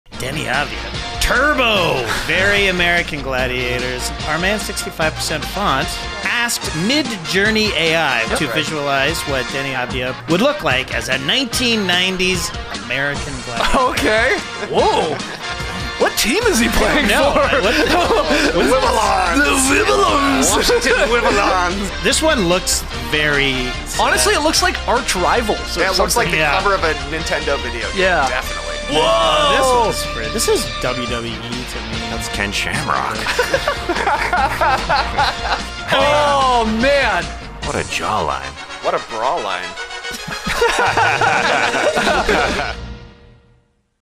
AI art turns Deni Avdija into Turbo, American Gladiator 🤣
We asked an AI art generator to visualize what ⁦Washington Wizards forward Deni Avdija⁩ would look like as a 1990s American Gladiator.